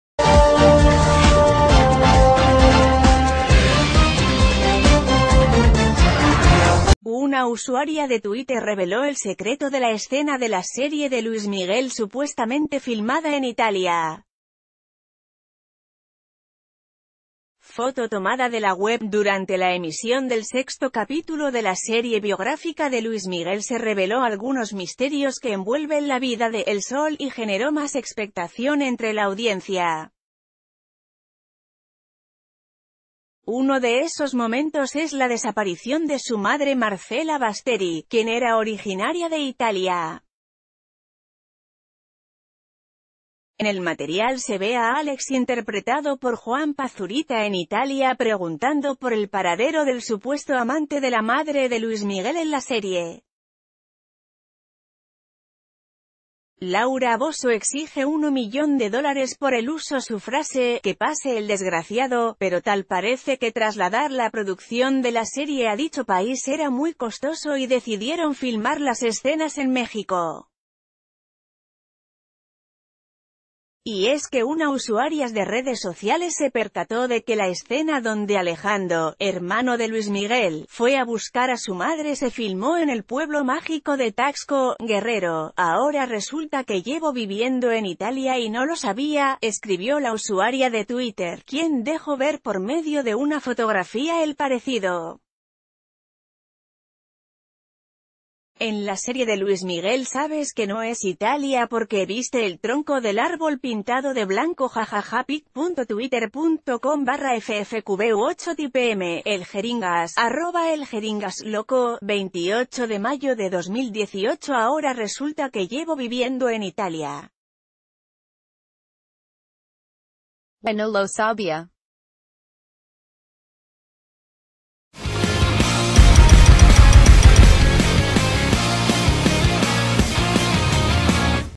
¡Nos engañaron! La serie de Luis Miguel no se grabó en Italia
Una usuaria de Twitter reveló el secreto de la escena de la serie de Luis Miguel supuestamente filmada en Italia. (FOTO TOMADA DE LA WEB)Durante la emisión del sexto capítulo de la serie biográfica de Luis Miguel se reveló algunos misterios que envuelven la vida de "El Sol" y generó más expectación ...